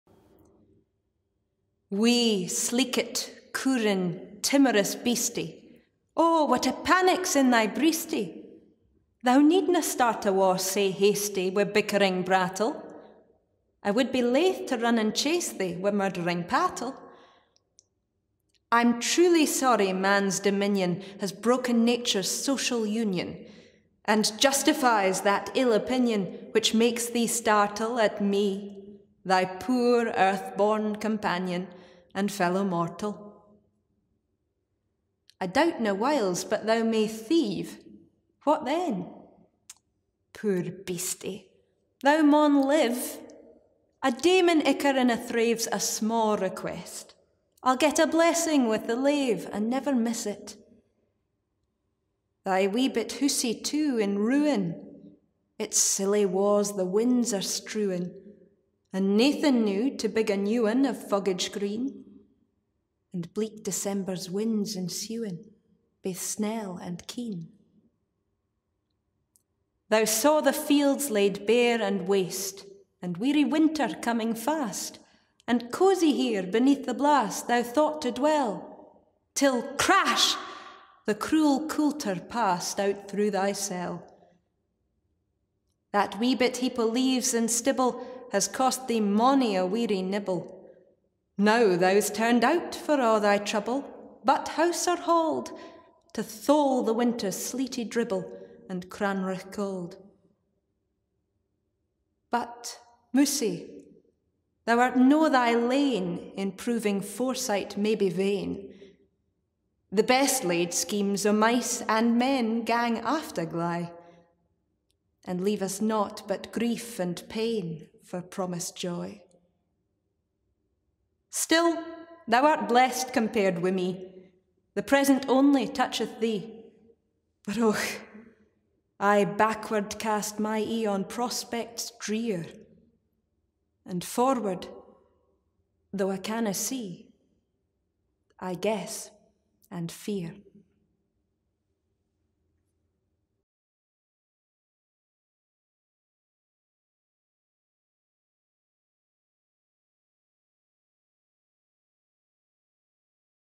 To a Mouse, Robert Burns
Scottish Actress, Eva Traynor, performs Robert Burns' 'To a Mouse'